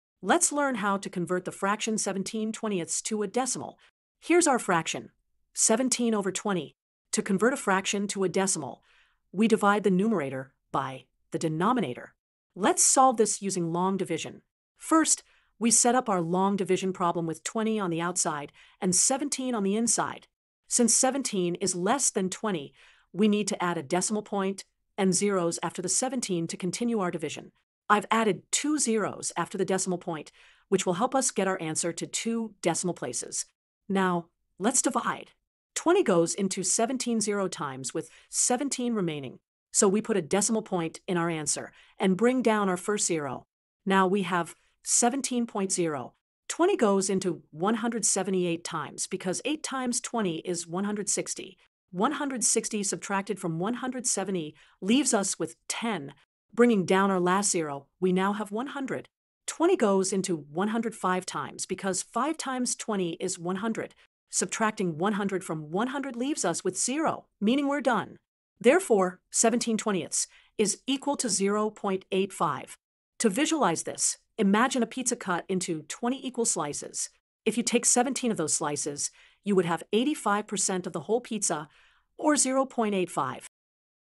Convert the Fraction 17/20 to a Decimal
How to Convert the Fraction 17/20 to a Decimal

1. **Divide the Numerator by the Denominator**:
   - To convert the fraction to a decimal, divide the numerator (17) by the denominator (20).
   - Perform the division: 17 ÷ 20.

2. **Perform the Division**:
   - 17 ÷ 20 = 0.85.

Final Answer:
- The decimal equivalent of 17/20 is **0.85**.